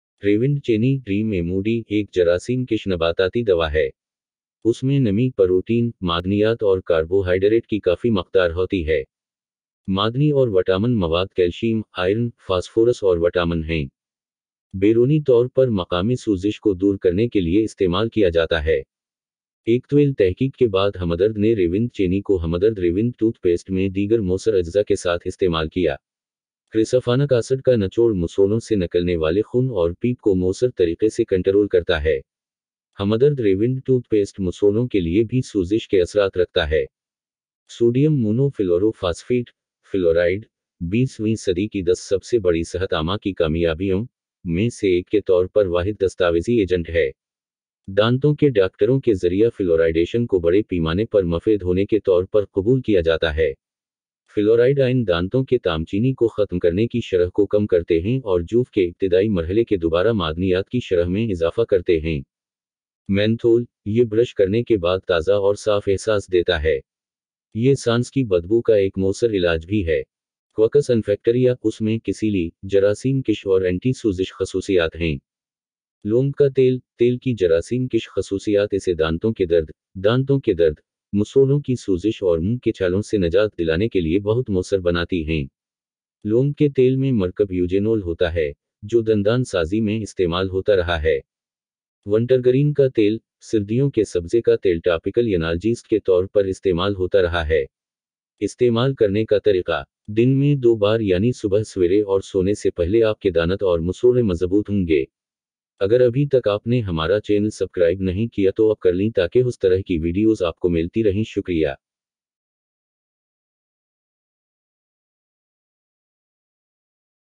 Revand Toothpaste Hamdard
Revand powder; Modern research confirms that Revand Chini (Rheum emodi) is a bactericidal botanical medicine. It consists of substantial amount of moisture, protein, minerals and carbohydrates.

The mineral and vitamin contents are calcium, iron, phosphorus and vitamin.

Applied to relieve local inflammation externally.

After a long research hamdard had used Revand Chini with other effective ingredients in Hamdard Revand toothpaste. The extract Chrysophanic Acid effectively controls treats the bleeding and pus oozing out from gum. Hamdard Revand toothpaste hasanti-inflammatory effects for gum too.

Sodium monofluorophosphate: Fluoride is the only well-documented agent as one of the “ten greatest public health achievements of the 20th century”. Fluoridation is widely accepted by dentists as being useful.The fluoride ions reduce the rate of tooth enamel demineralization and increase the rates of remineralizationof the early stage of cavities.

Menthol: It gives fresh and clean sensation after brushing. It is also an effective bad-breath remedy.

Quercus infectoria; It has the astringent, antiseptic and anti-inflammatory properties.

Clove oil: the germicidal properties of the oil make it very effective for relieving dental pain, tooth ache, sore gums and mouth ulcers. Clove oil contains the compound eugenol, which has been used in dentistry.

Oil of Wintergreen: Oil of wintergreen has been used as topical analgesic.